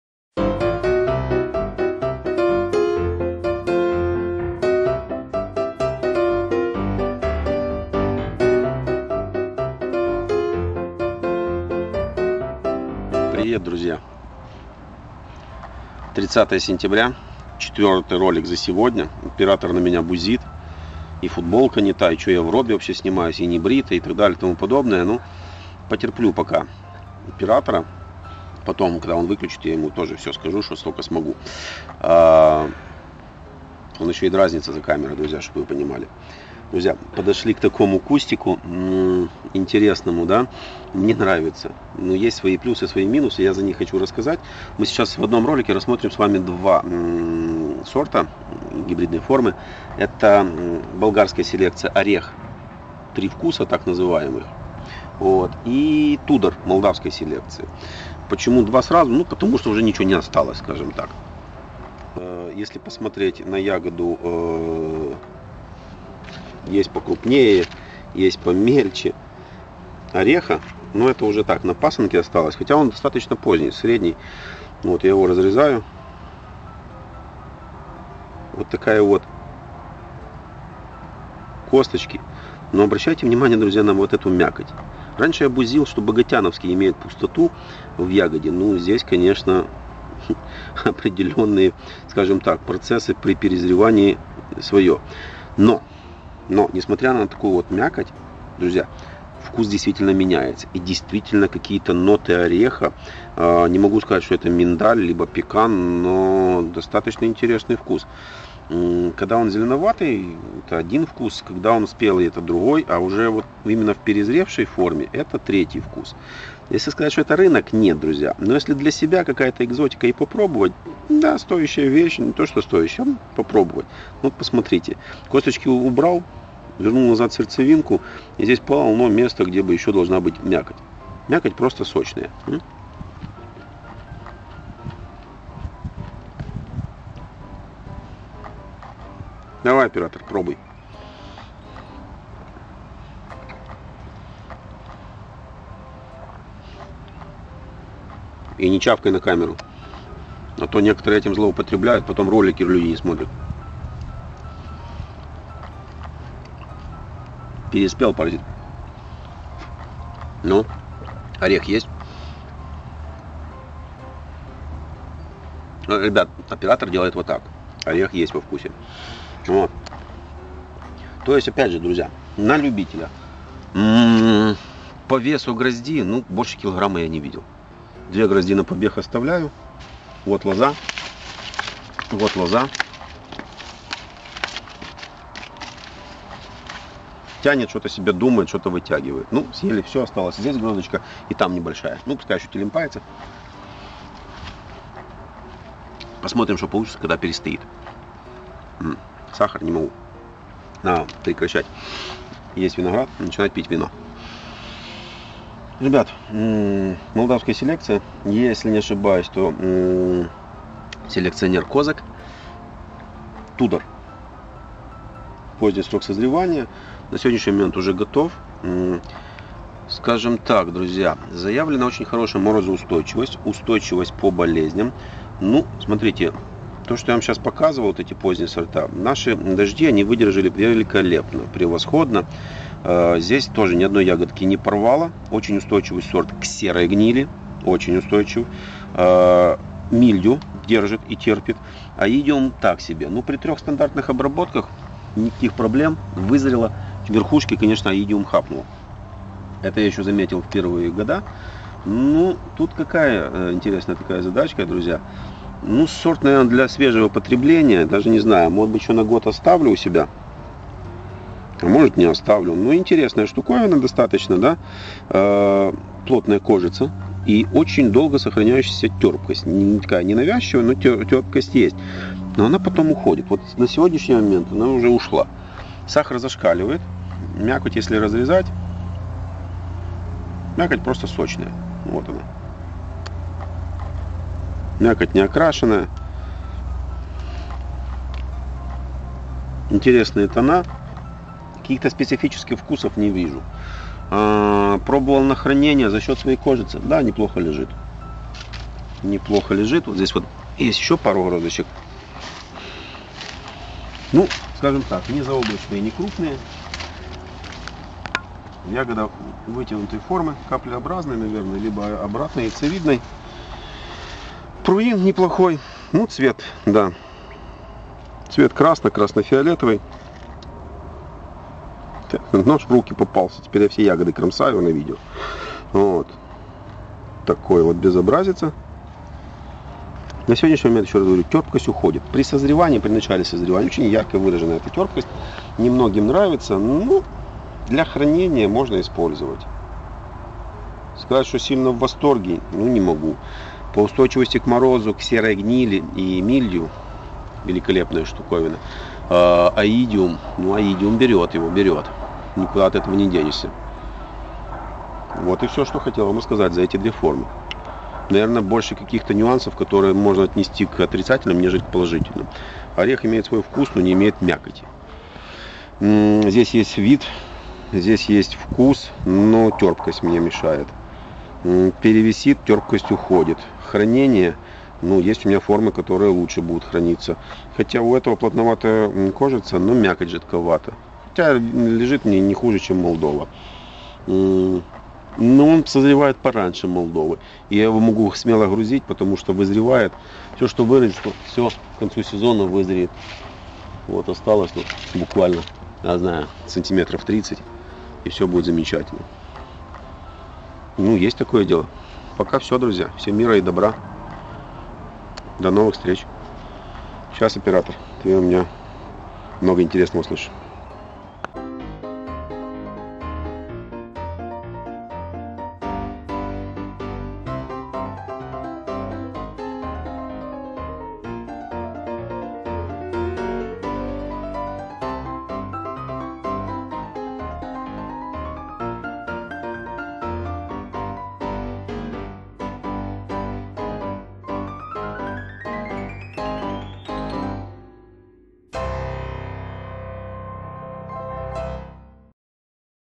Орех и Тудор 2019
Характеристики  гибридных форм Орех и Тудор  (в наших условиях).
Питомник "BurVon": делимся опытом, реализация саженцев винограда, более 300 сортов. Усатово, 5 км от Одессы.